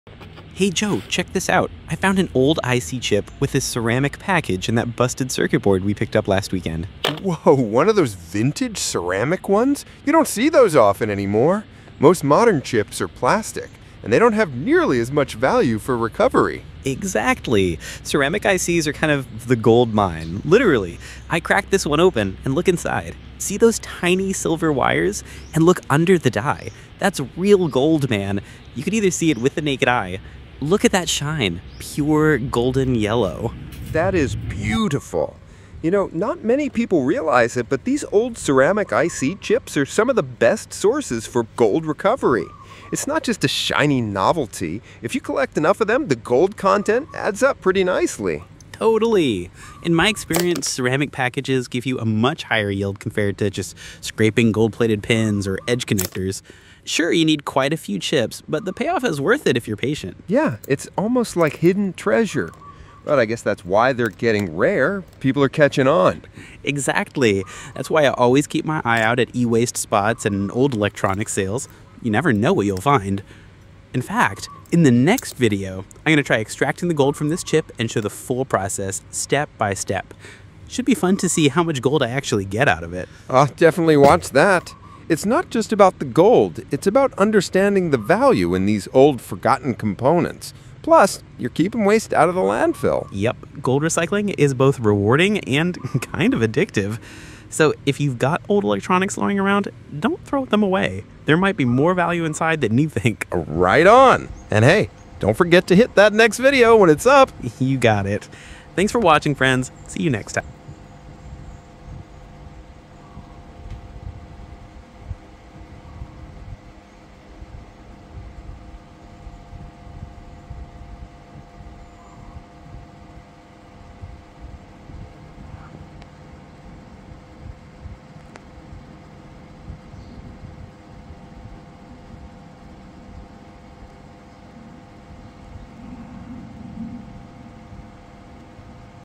💛 “Hidden Gold Inside Vintage Ceramic IC Chips – A True Gold Mine!” 💻✨
Hey Joe, check this out! Inside old ceramic IC chips lies real gold — tiny shimmering wires and golden layers under the die. Unlike modern plastic chips, these vintage ceramic packages hold valuable amounts of gold, making them some of the best materials for gold recovery and e-waste recycling.

In this video, we uncover:
🔹 Why ceramic IC chips are rich in gold
🔹 How to identify valuable vintage chips
🔹 The difference between ceramic and plastic ICs
🔹 Why gold recovery from old electronics is both profitable and eco-friendly
🔹 A sneak peek at the next video — step-by-step gold extraction from these rare chips!

♻️ Don’t toss your old electronics — there’s hidden treasure inside!